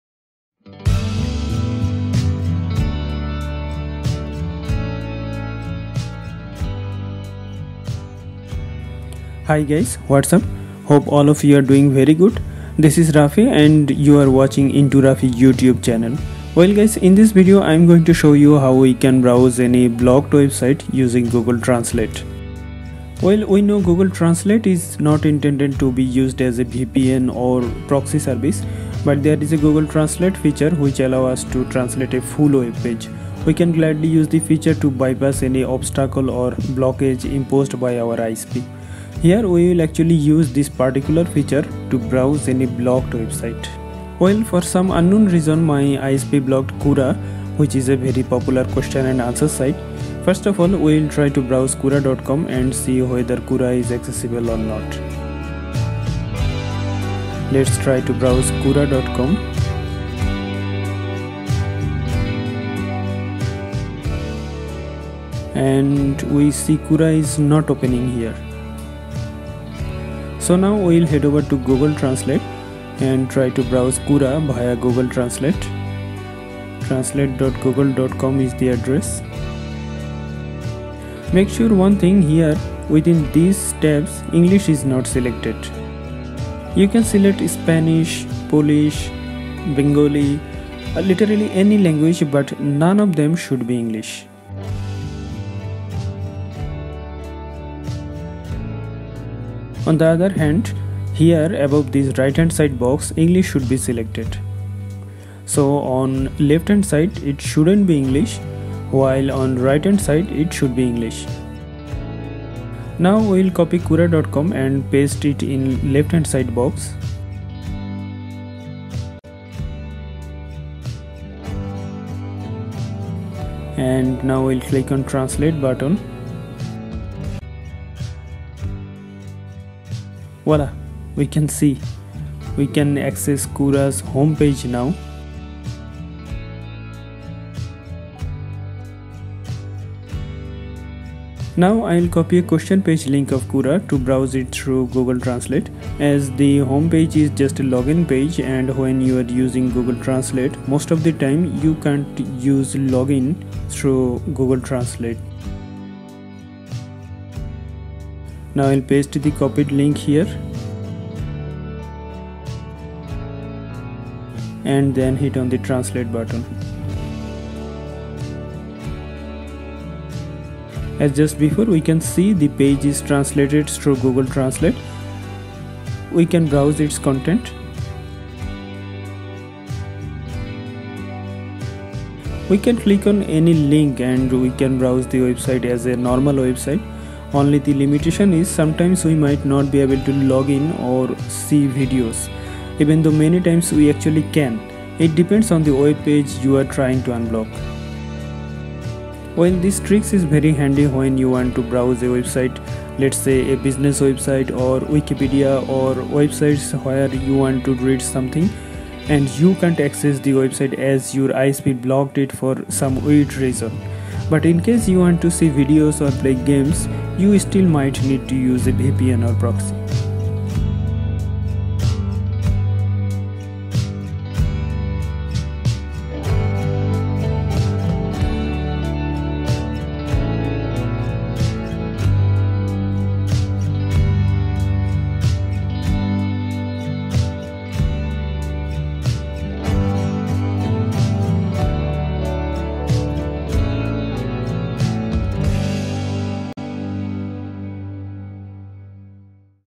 How to Access/Unblock/Open Blocked Websites With Google Translate - Google Translate as Proxy or VPN
How to open/access/unblock any blocked website with Google translate is shown in this video. Google translate can translate an entire webpage to a given language. We use the feature to bypass filter and unblock blocked website. Here google translate is being used as a proxy or VPN. Here are the steps to browse blocked websites...

3- Paste the copied URL into left hand side box of google translate. Make sure language of the box is any language rather than English.
4- Hit translate button from above of the right hand side box. Make sure here English is selected from top of the right hand side box.

Done! The blocked website will be unblocked via Google Translate.

No VPN, Proxy, IP, Software, Download, tor browser needed. You need to use only Google Translate to bypass any blockage on your favorite website. In schools often WiFi networks block some specific websites like Reddit or some social medias. But truth is often we need to access these forums, social media sites to access our study relevant information. As often you can not install any software on school, college or University computer you can easily use Google Translate website to access your intended website.

Please be ensured this is not any kind of hack or you are not going to do any illegal task. Just use the method to access important website and read contents. You probably will not be able to see any YouTube videos. But in some cases you will be able to see streaming videos too.

I personally found Google Translate very handy when I was traveling to China and I noticed there Wikipedia is blocked. I used Google Translate to bypass the obstacle and was succeed to unblock Wikipedia using the exact same method I described here.